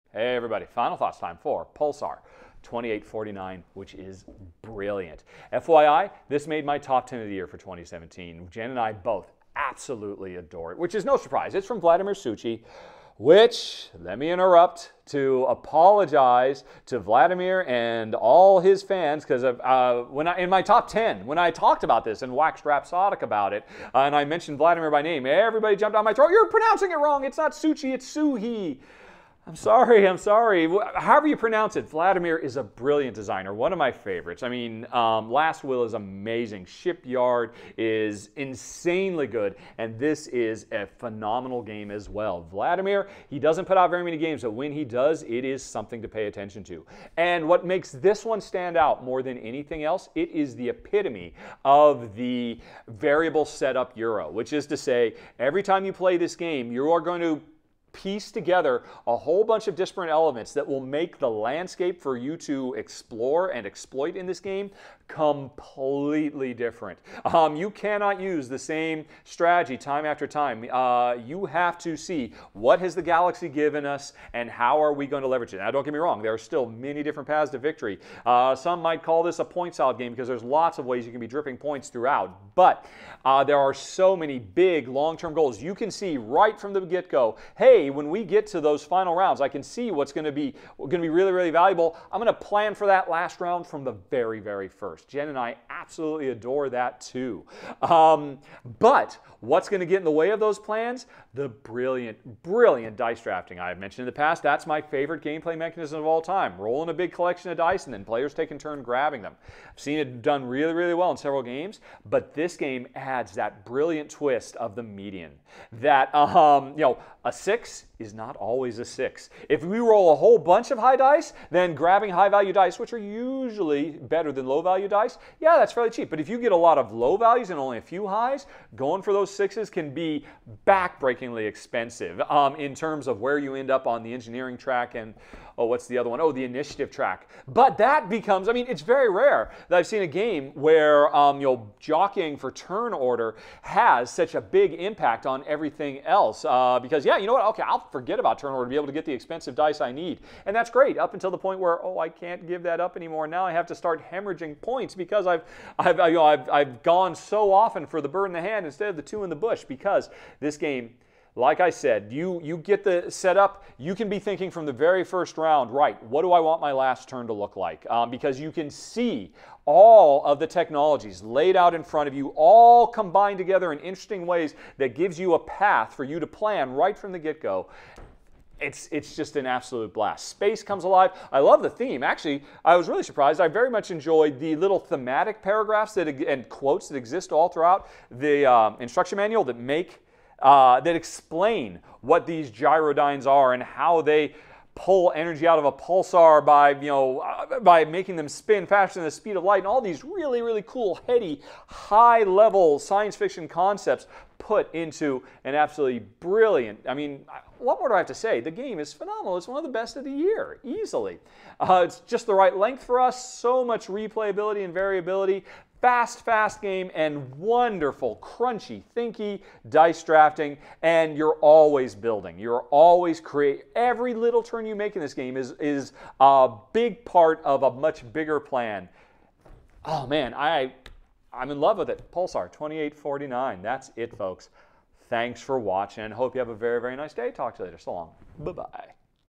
Pulsar 2849 | Rahdo's Final Thoughts
A video outlining gameplay for the boardgame Pulsar 2849.